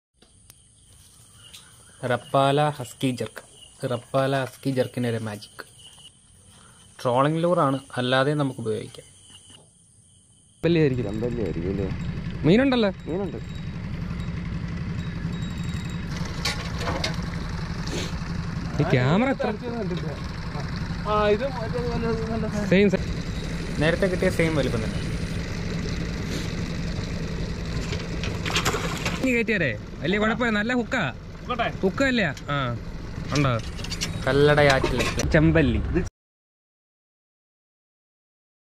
Mangrove jack | Red snapper | chempalli | ചെമ്പല്ലി
a mangrove jack catch from our river
using husky jerk by rapala trolling lure